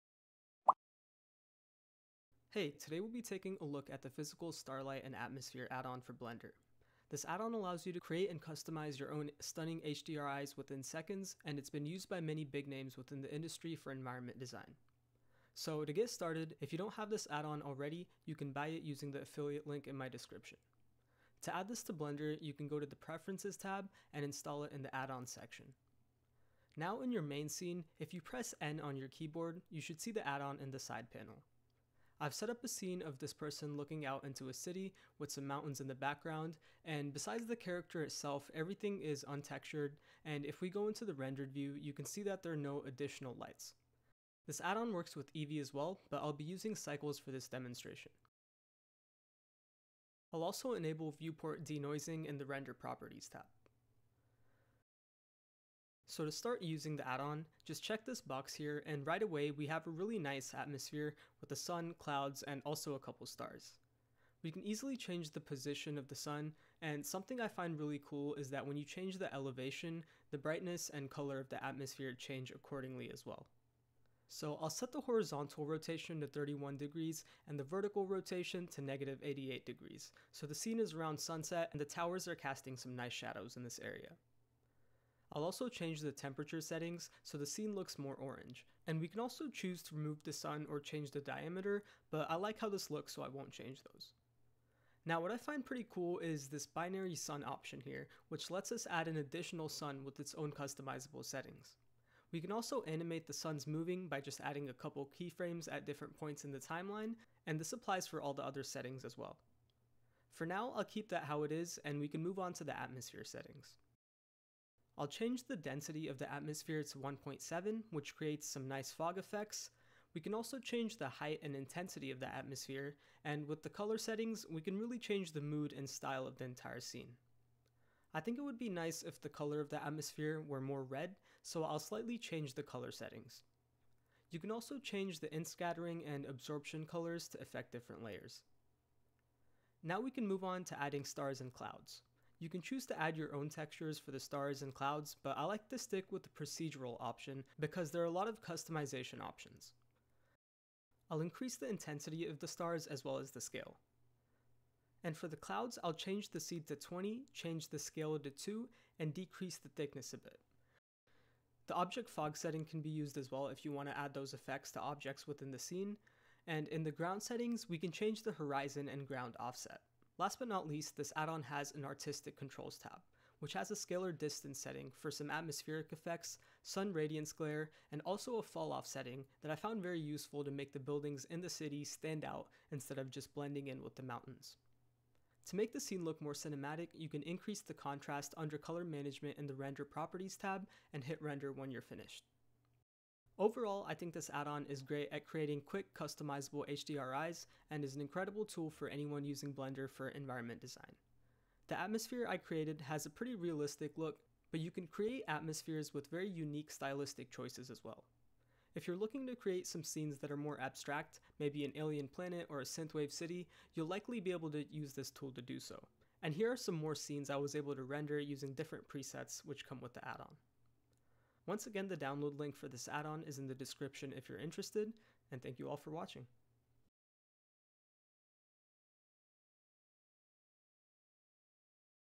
Custom HDRIs with PSAA - Blender Add-On
In this video, I showcase the features of the Physical Starlight and Atmosphere add-on for Blender.

0:00 - Introduction
0:55 - Using the Add-On
3:13 - Final Thoughts